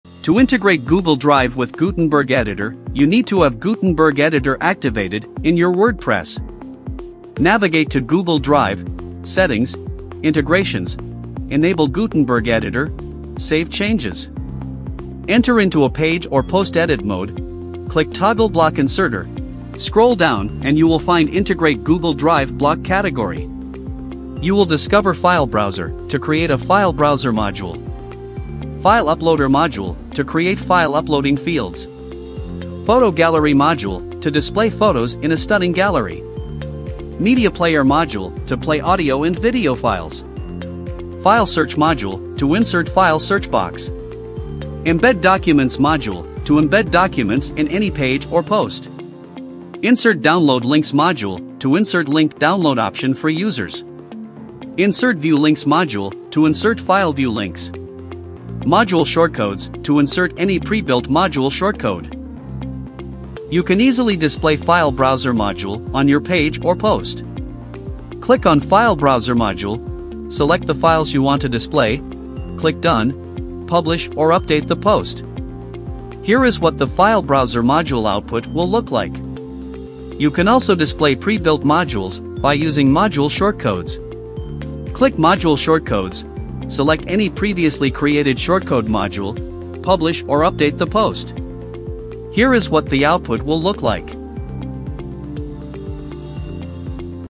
How to Use Integrate Google Drive With Gutenberg Editor
Using Integrate Google Drive Plugin with Gutenberg Editor is easy. Here we present a video tutorial how can you do the complete process in a matter of time.